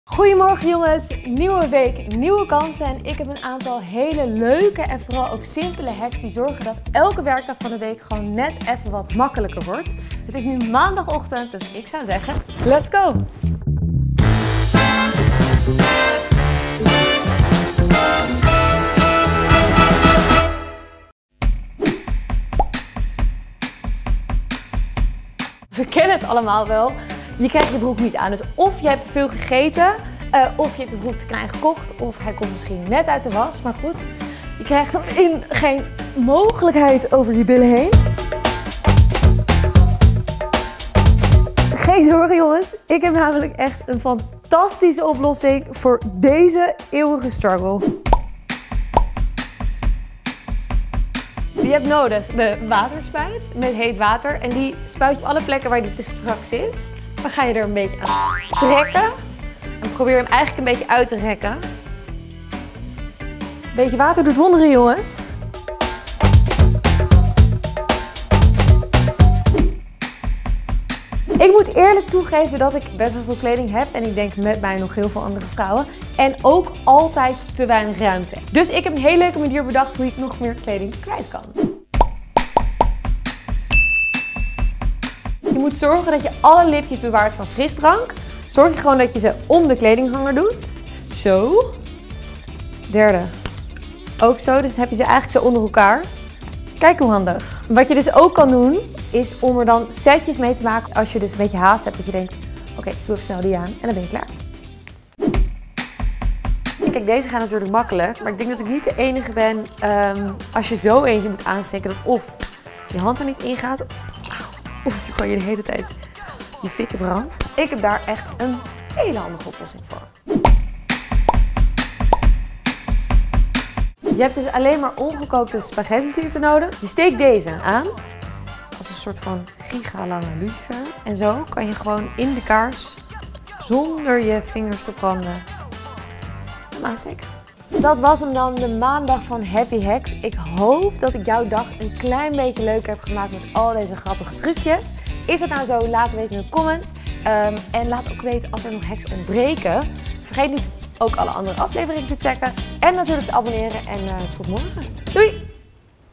MET DEZE HACK KRIJG JE DIE TE STRAKKE SPIJKERBROEK WEER AAN - &C
Een nieuwe week betekent nieuwe ronde en nieuwe kansen. Samen met Iris Enthoven en Nivea geven we je voor elke dag van deze werkweek drie Happy Hacks om jouw dag net wat makkelijker te maken.

Passen jouw spijkerbroeken ineens niet meer? Misschien heb je iets te veel gegeten, een verkeerde maat gekocht of de broek te heet gewassen. Geen paniek, wij laten zien hoef hem alsnog aan krijgt. En nee, je hoeft er geen pond voor af te vallen.

Nu deze te kleine spijkerbroeken niet langer in de prullenbak verdwijnen, zal je vast wat meer ruimte in je kledingkast nodig hebben. Wij geven de ultieme tip waardoor je drie keer zoveel kleding in je kast kwijt kunt.

Ook leren we je om een kaars aan te steken zonder je handen te branden. Wij durven onze handen in het vuur te stekken dat deze hack ook jou zal helpen.

Kijk de video met alle hacks en maak er een mooie dag van! Happy Hacks. Happy Feeling.

Camera:
 Sanne Bakker, Anneke Bleeker & Daphne Ooms
Montage:
 Sanne Bakker